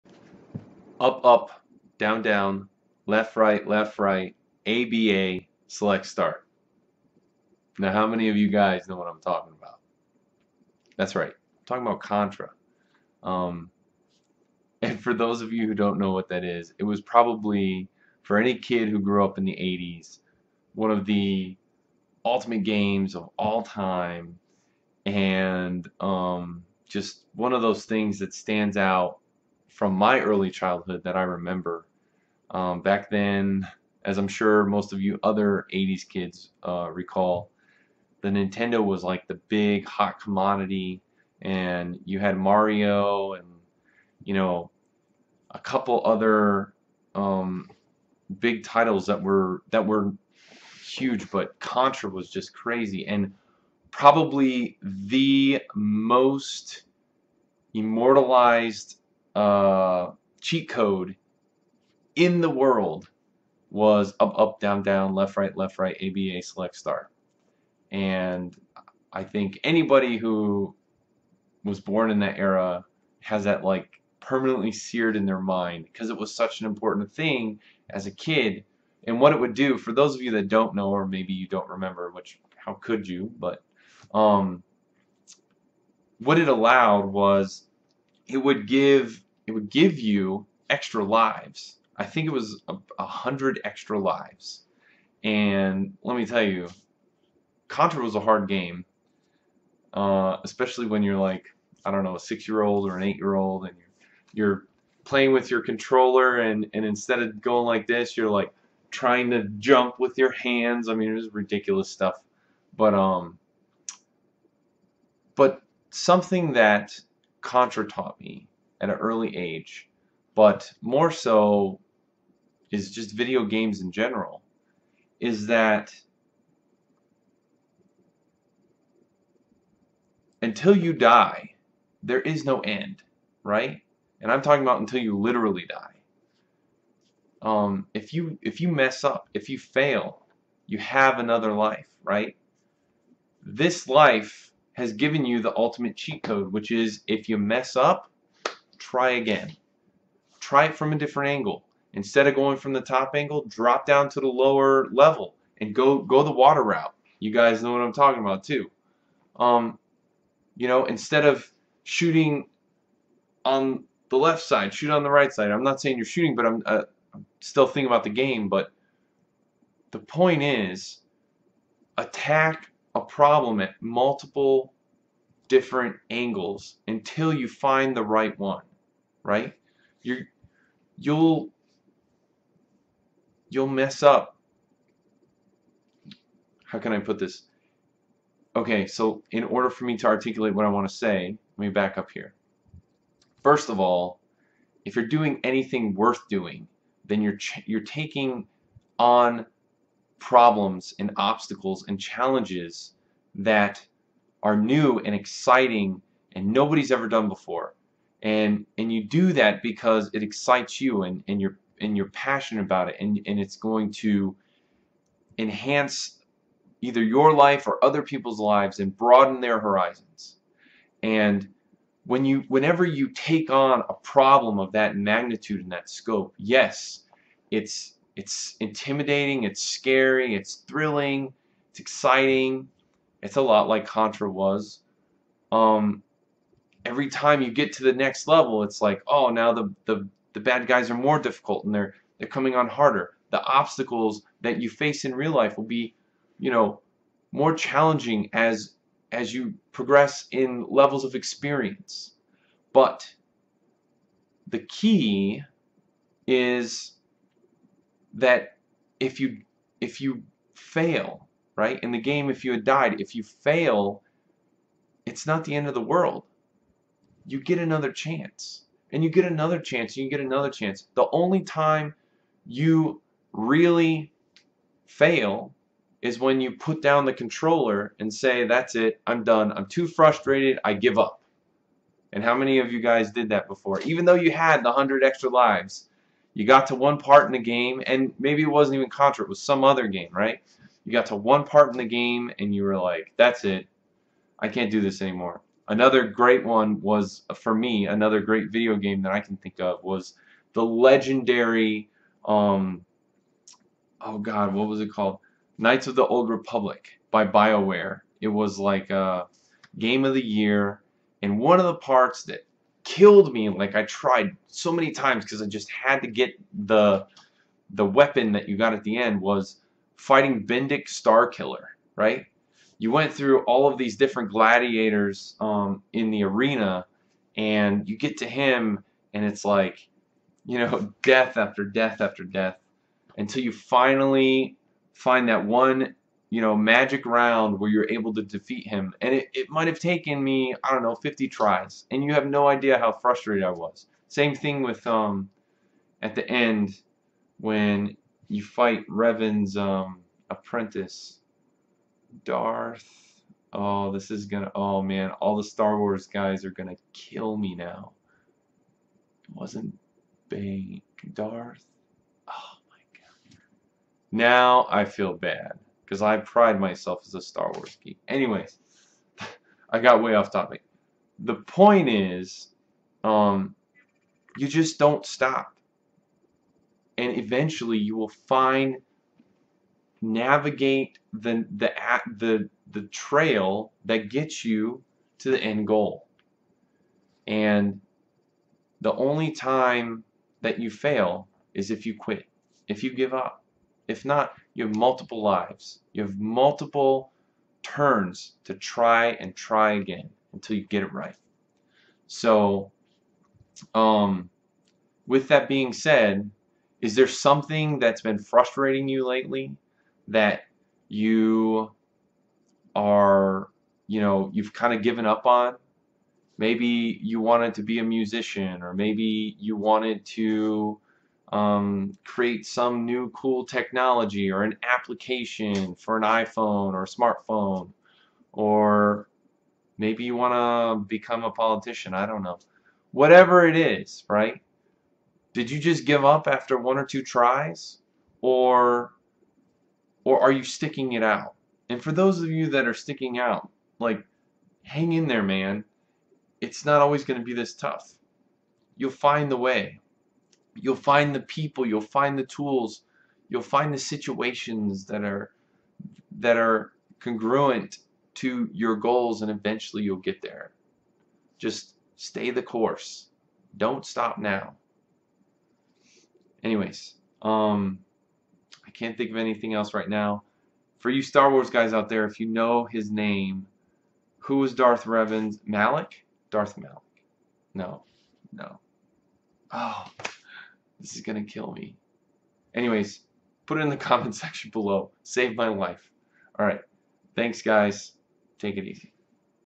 What Contra and Knights of the Old Republic taught me about life--Polymathics
In this video I explain how Contra and Star Wars Knights Of the Old Republic can give you a different perspective on failure.